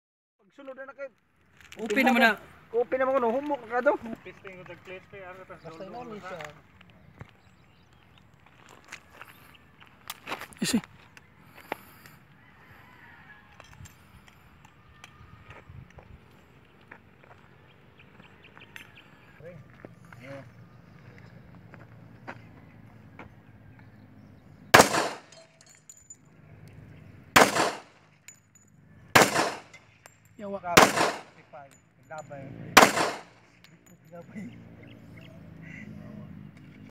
Alicia ps pnp pers practice firing#firearmprofeciency#markamnship
Alicia mos personnel conducted firearm proficiency markmanship